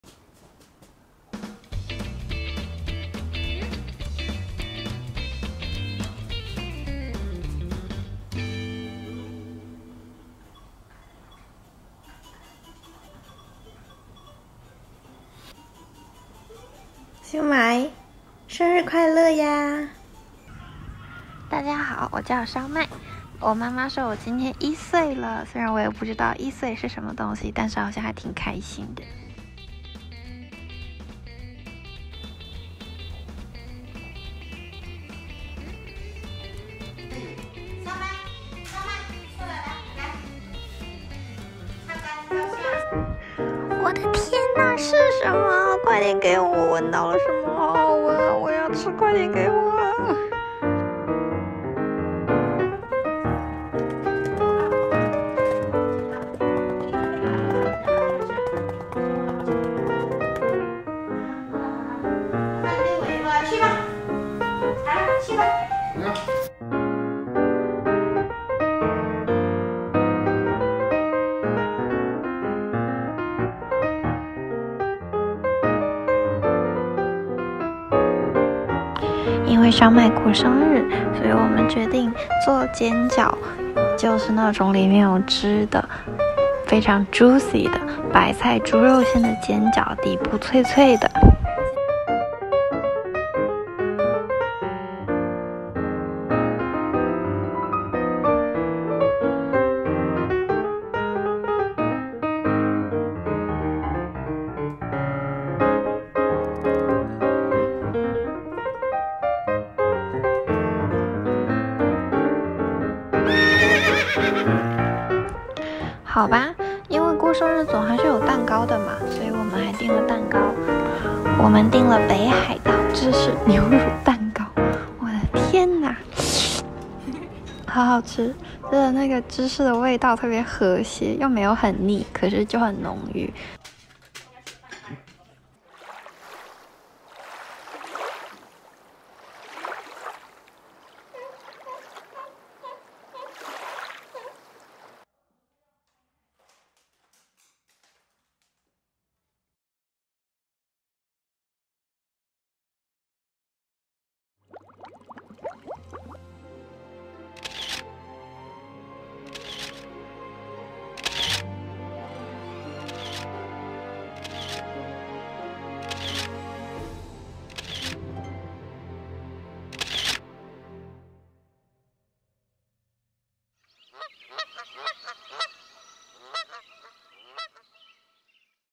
我的烧麦过生日啦-猫咪生日餐-生日煎饺蛋糕-肥肚肚
猫咪烧麦是我的大儿子
Sphynx cat SuiMaak is my 'first born' son
一根毛一块肉的拉扯长大
I tried my best to grow him up
虽然他没毛
终于长大到一岁啦
As for now it is his first birthday
于是我们决定犒劳一下自己
We decided to treat ourselves
给他做了马鲛鱼虾仁羊肉生日餐
He was offered by mackerel, shrimp and lamb as his birthday main course
我们自己煮了煎饺还吃了蛋糕
We tasted fried dumplings and Hokkaido milky cheese cake.